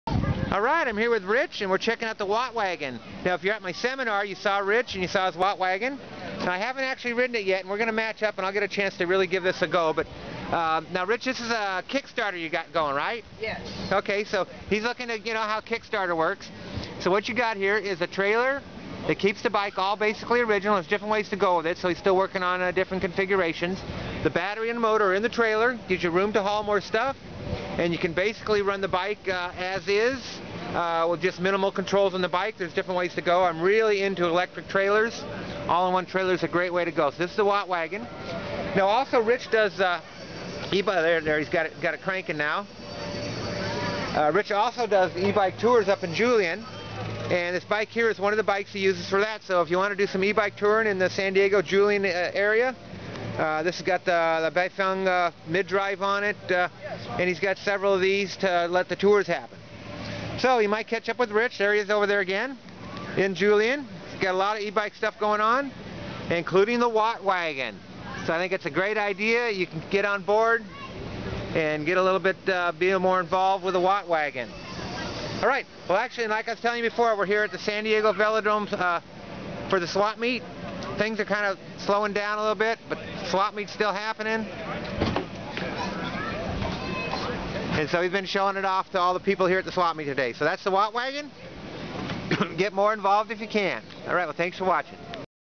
Wattwagon---Electric-assist Trailer Kickstarter
I will get a chance to give the Wattwagon a burn-down test soon. It made a showing at my E-bike seminar last month and now at the San Diego Velodrome swap meet.
Check it out on KickStarter.